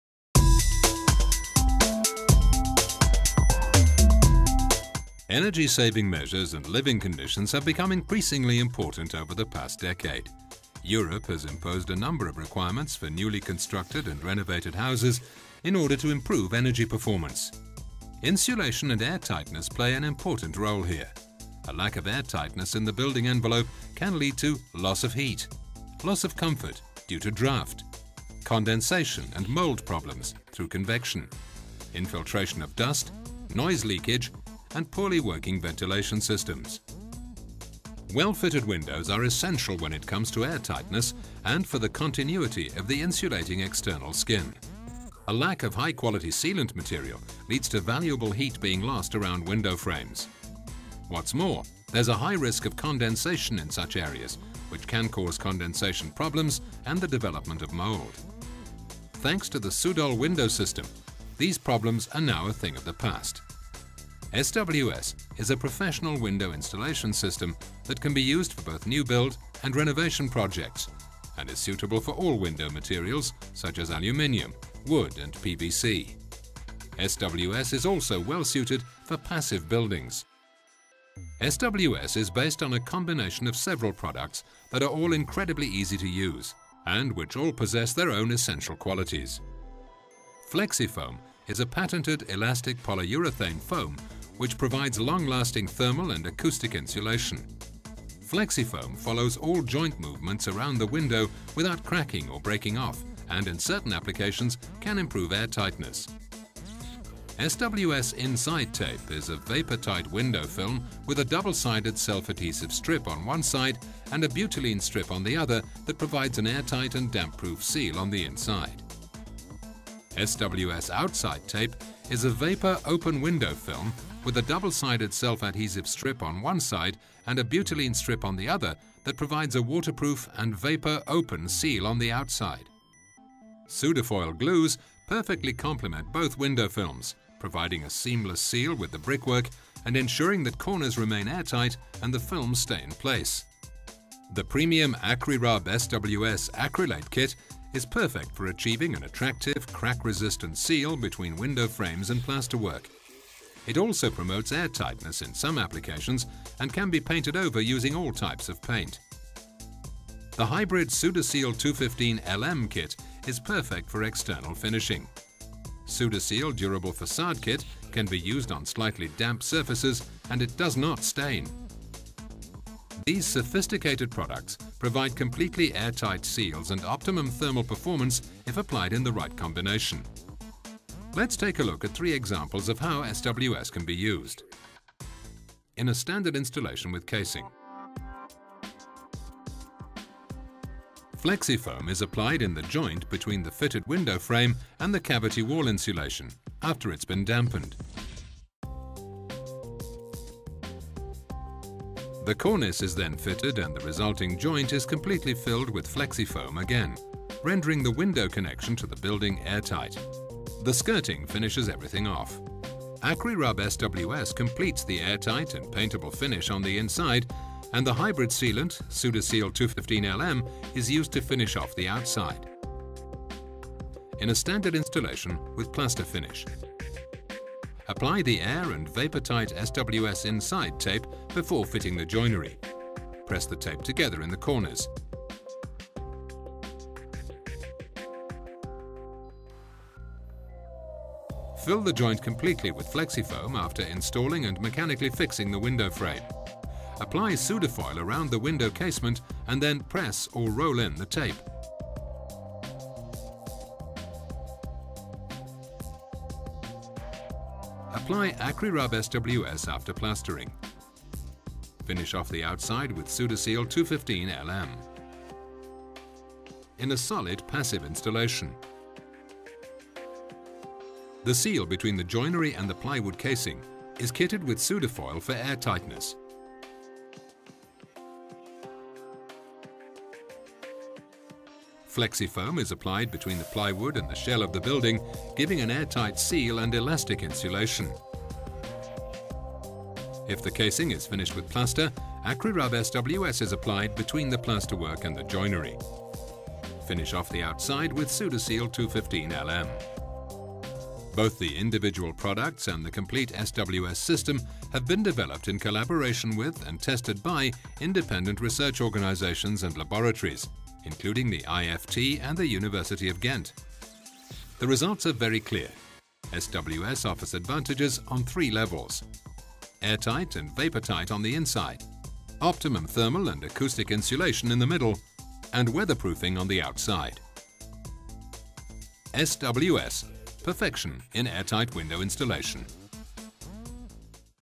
Soudal Window System | Professional window joint sealing system
Soudal Window System:
The Professional Window connection system. SWS is a balanced combination of existing and new products to seal connection between window frames and building structure.
Window installation according to EPB.

Soudal Window System offers a solution for the increasingly stringent expectations and requirements regarding the energy performance of buildings, by combining (certified) high-quality products for a long-lasting, well insulated, airtight and weatherproof seal around window & door connections. Through its diversity in products (sealants, PU-foams, pre-compressed tapes, (liquid) membranes and self-adhesive tapes) SWS can always offer the right combination and solution regardless of the building or installation method!